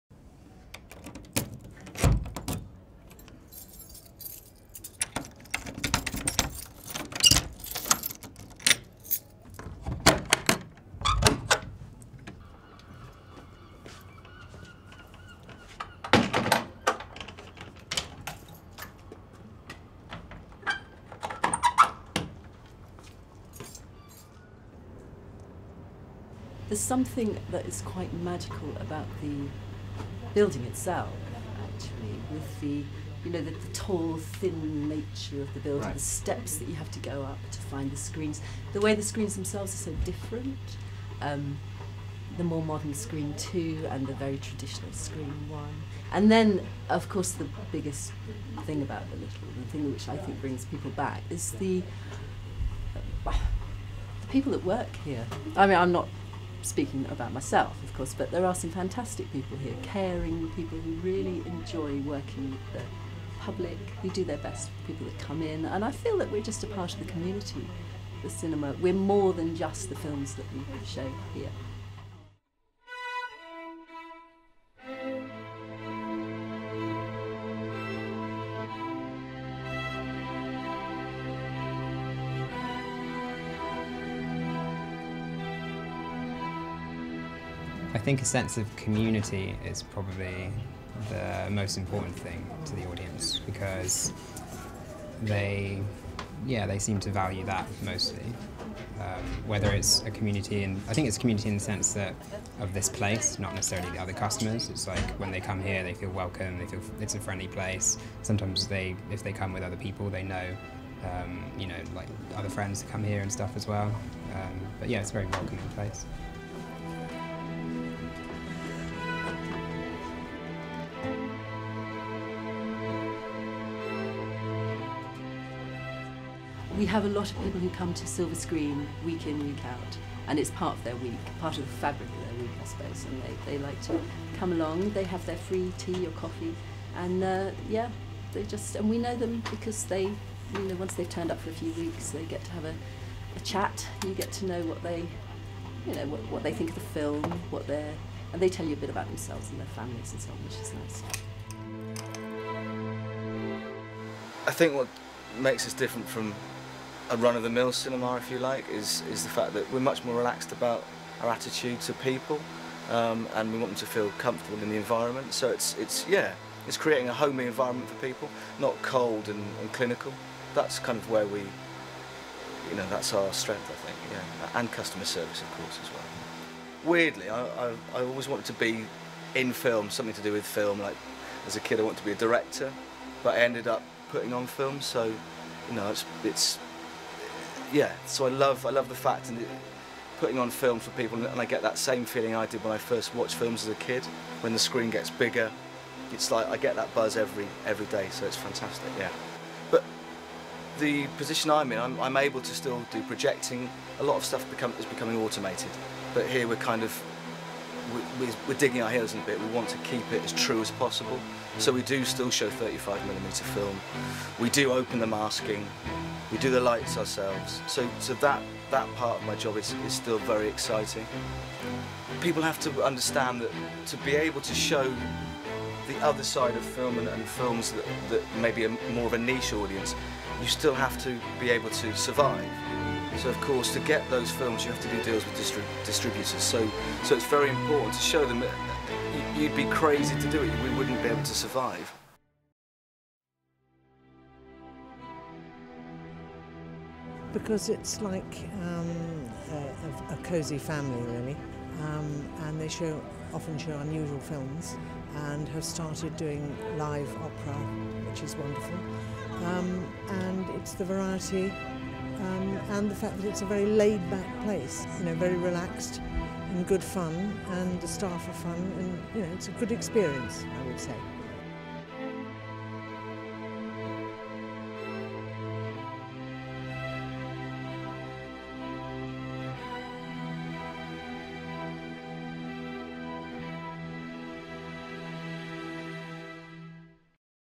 At the Pictures
A visual documentary short on 'The Little Theatre' located within the city of Bath.

This project was part of the Single Camera Production assignment at Bath Spa University